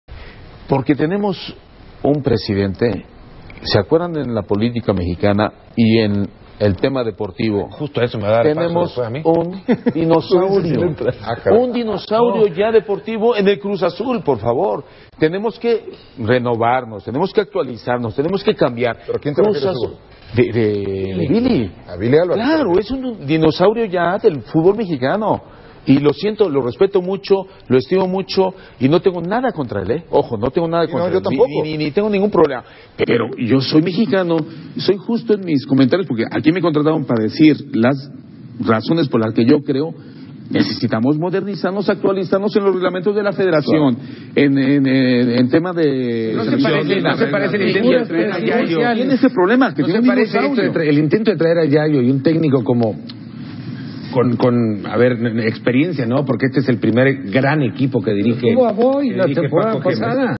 "Billy Álvarez es un dinosaurio del Fútbol Mexicano": Hugo Sánchez
ÚNETE SI ERES AZUL DE CORAZÓN